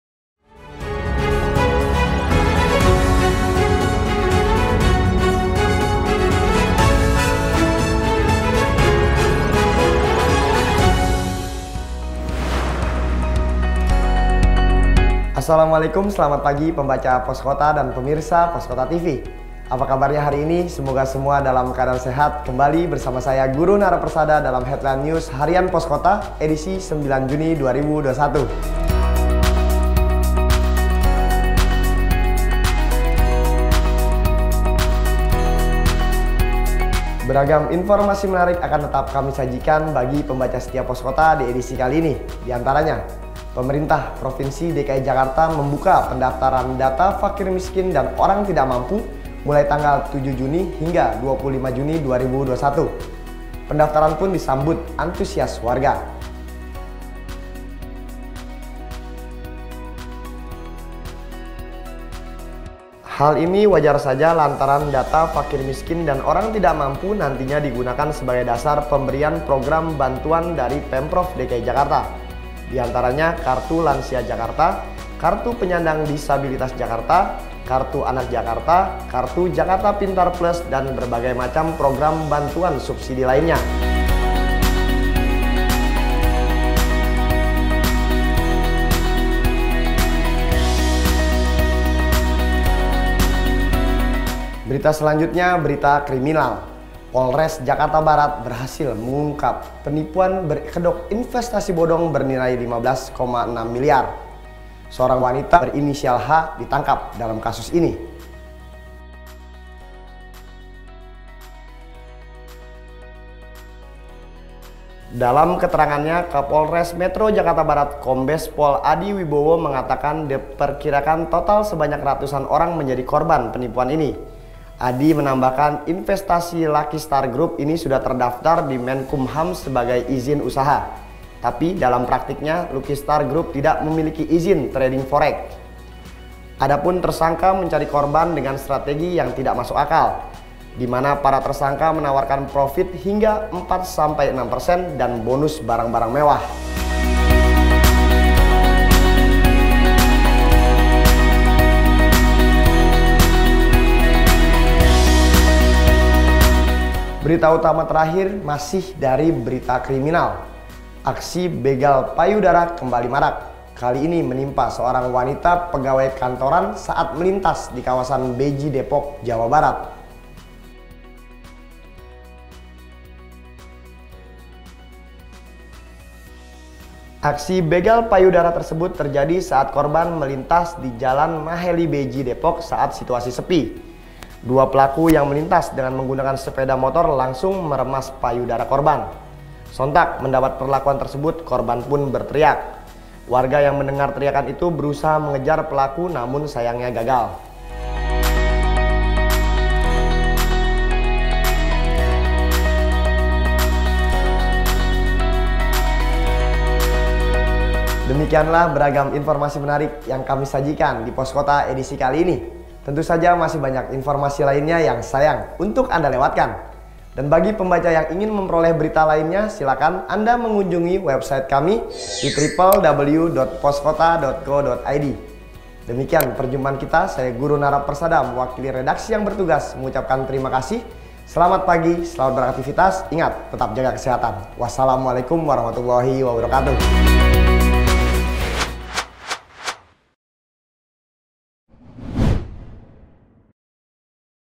HEADLINE HARIAN POSKOTA EDISI RABU 9 JUNI 2021
Harian Poskota menyajikan beberapa topik menarik di antaranya Warga pendaftar data Fakir Miskin dan Orang Tidak Mampu (FMOTM) membludak, penipuan berkedok investasi bodong bernilai Rp15,6 miliar di Jakarta Barat terungkap, perempuan pegawai kantoran di Depok jadi korban begal payudara.